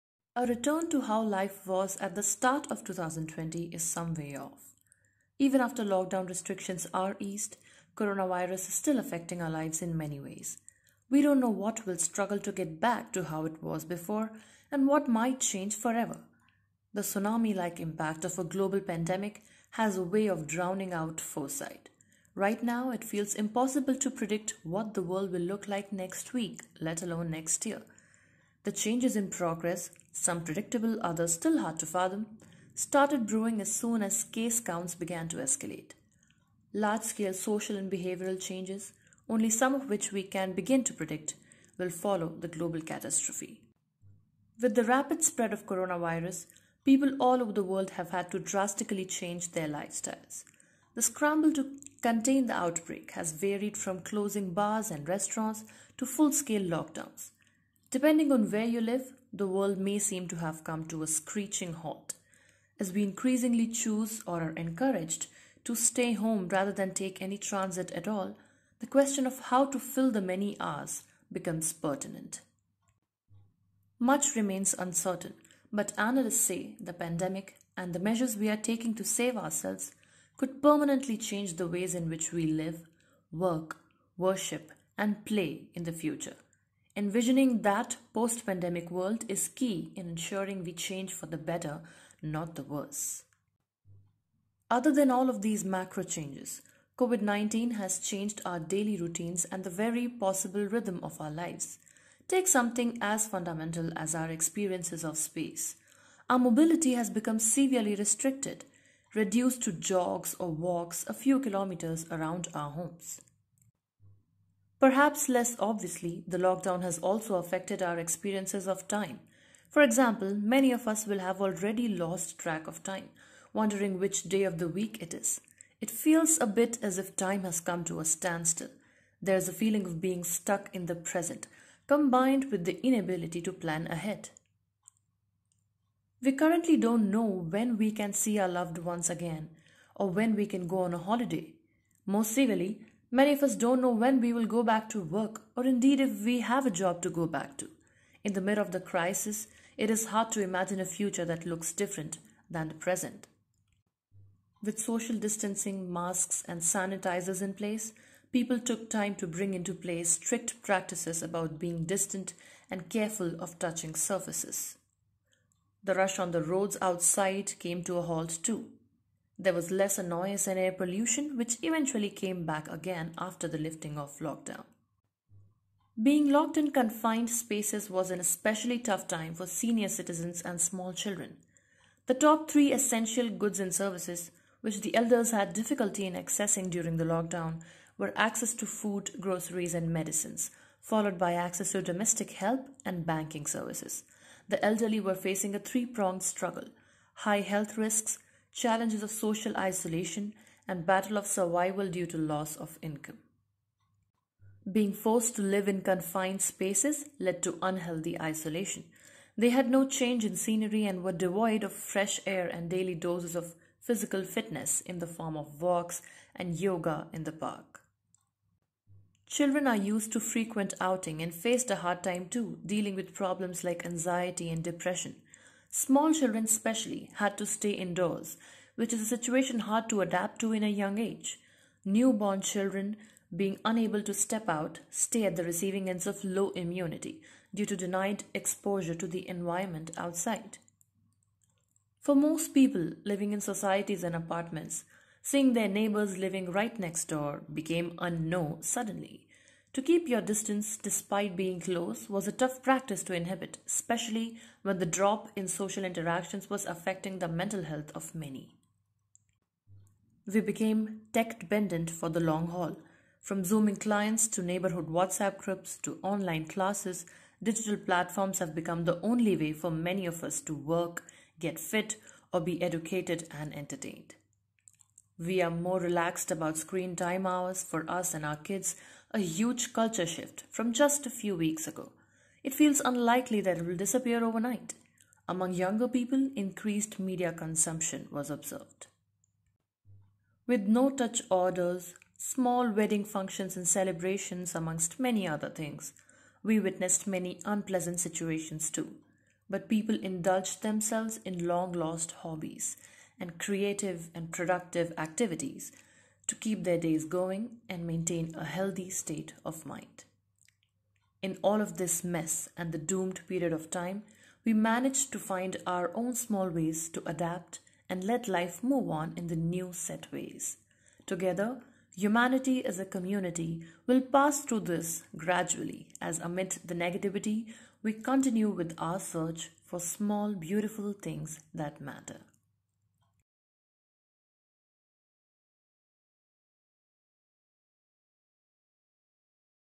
How the COVID19 Pandemic Changed Lives and Daily Routines of People | Anushka Pandit
A photo feature and audio story that walks through the changes people across the world went through after having gone through the COVID19 pandemic induced lockdown and how their daily routines were affected by the same.